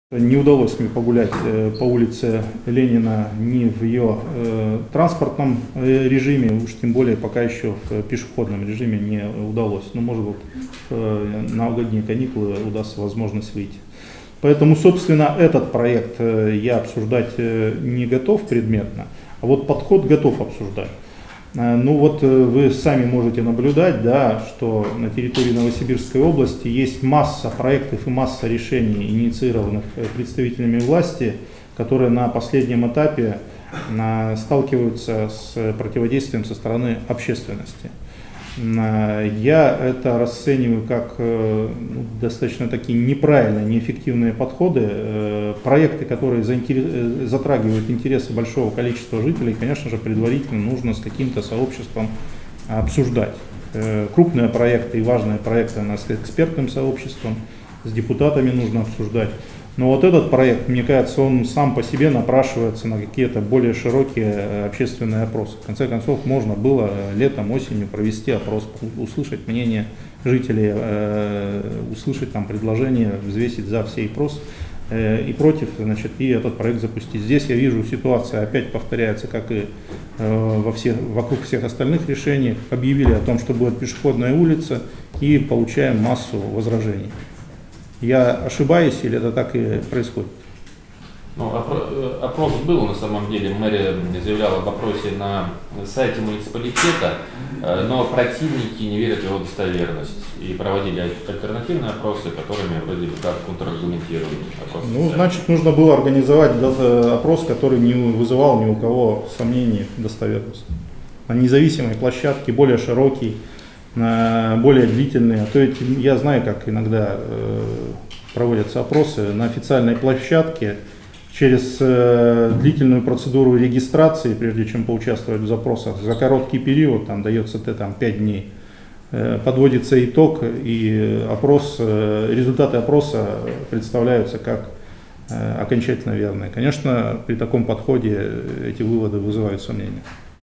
Андрей Травников об улице Ленина
Травников о пешеходной улице Ленина: «Это неэффективные подходы» Врио новосибирского губернатора считает, что такие большие проекты, как пешеходная улица Ленина, стоит более широко обсуждать с общественностью. В противном случае, будет нарастать конфликтная ситуация.